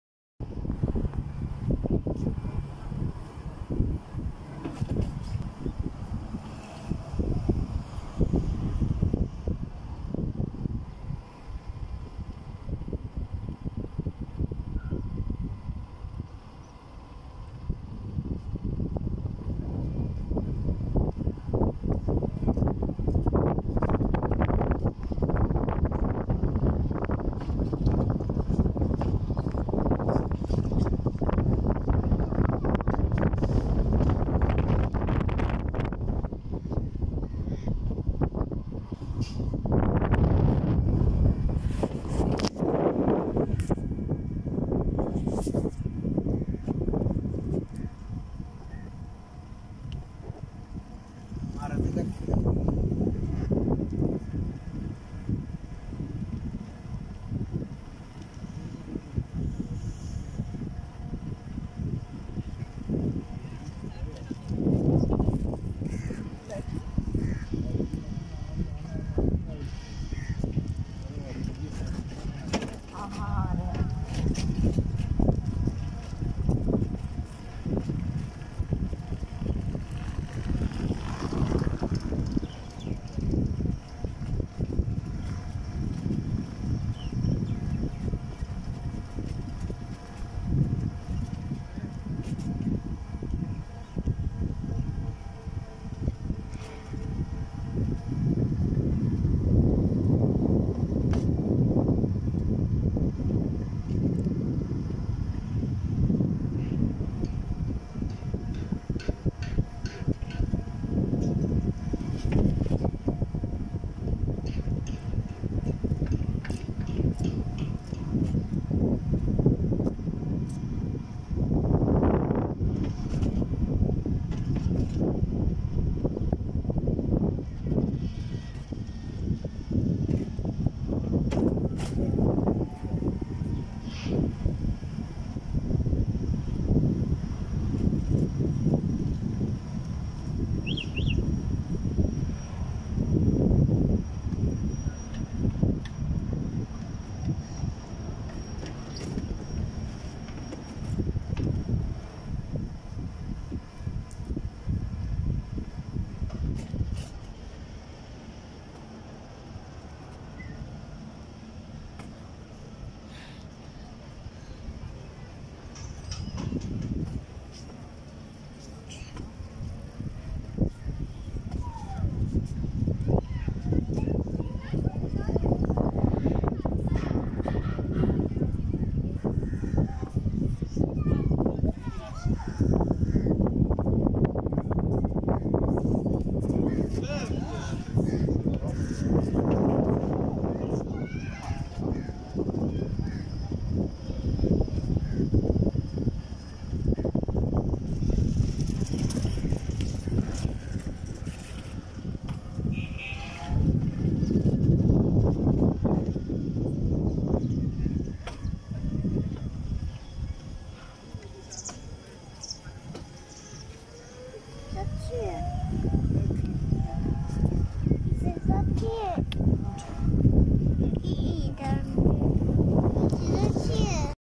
RIDE WITH ME | walk with me through beautiful KISH ISLAND| miniseries
RIDE with me on a bike along the beach of beautiful KISH ISLAND located at the southren part of IRAN.
The land gets rather modern and as the island moves away from the main part of the country/land, a different culture is also observed.